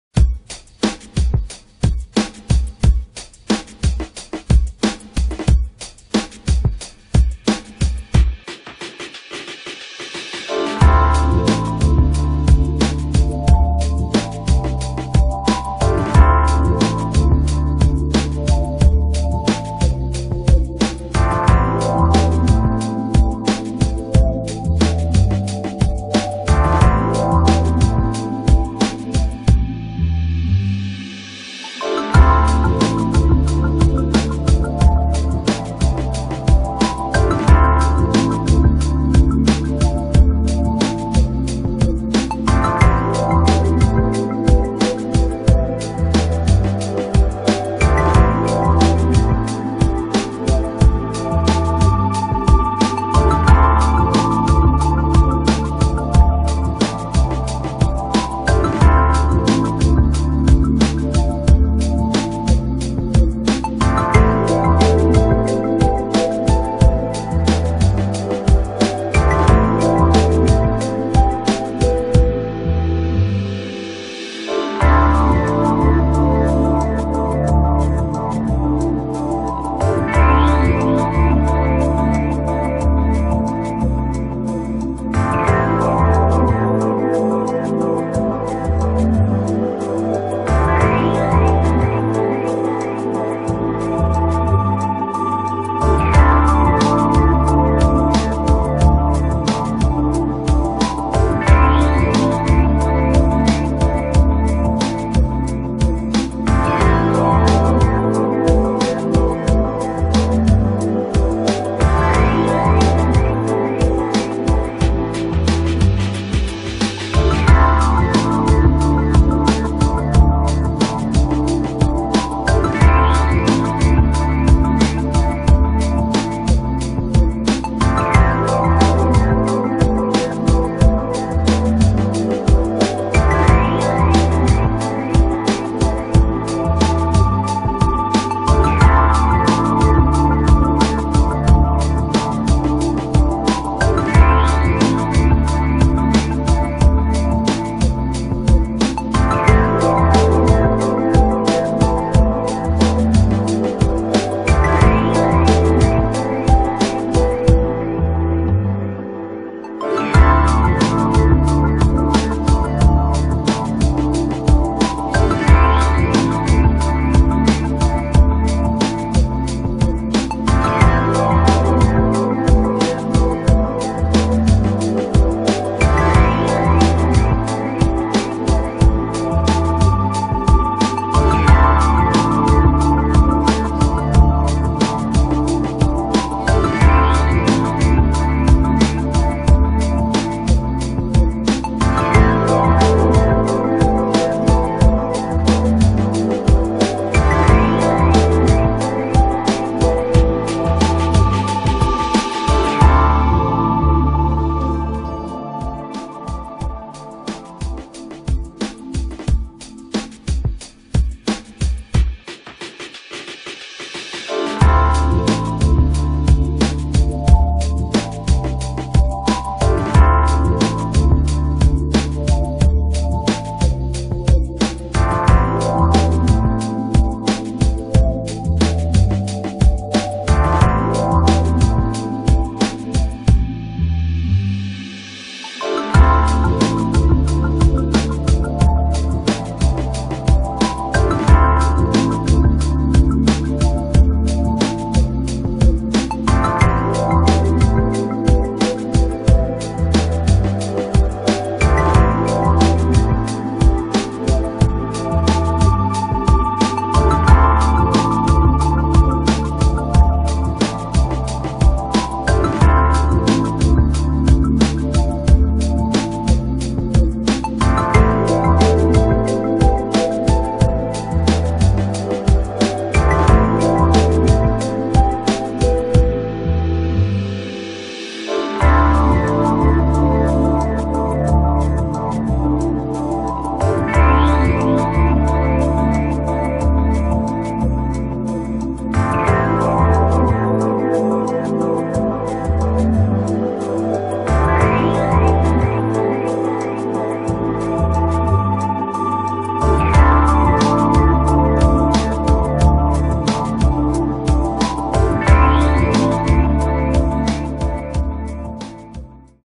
BERG Safety Net Comfort assemble video
This enclosure has nice clean lines with straight poles covered with a smart black sleeve which has foam inside to prevent little fingers picking at the foam! The net attaches at the top via a click system and bungee for added flexibility.  The enclosure poles connect to the legs of the trampoline via a U bolt for strength.  The trampoline entrance is via a self-closing entrance with a green trim to clearly show users the way.